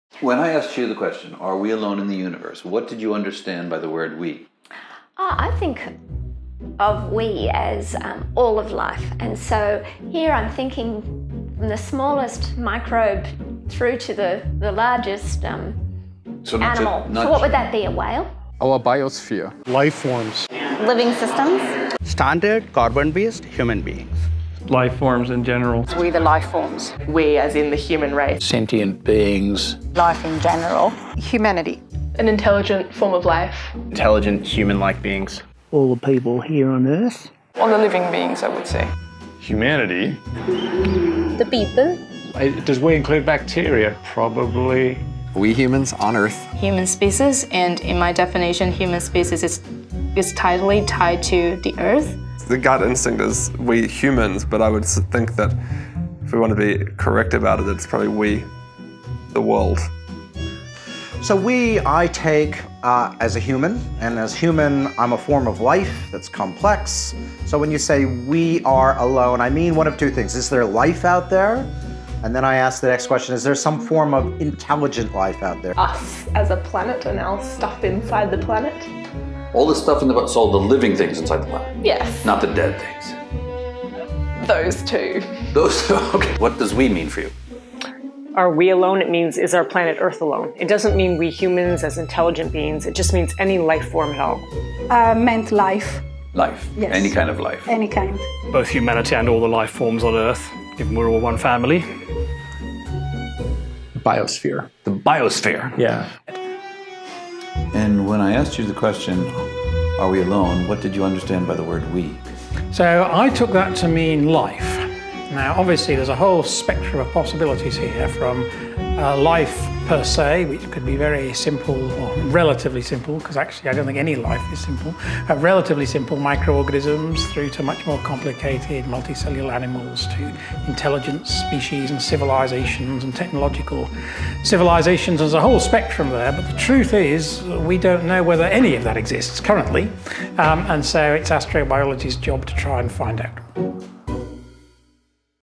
1.1.5 Who is "We"?
In the question "Are We Alone?", who is "We"? We humans? We animals? We the life forms on Earth?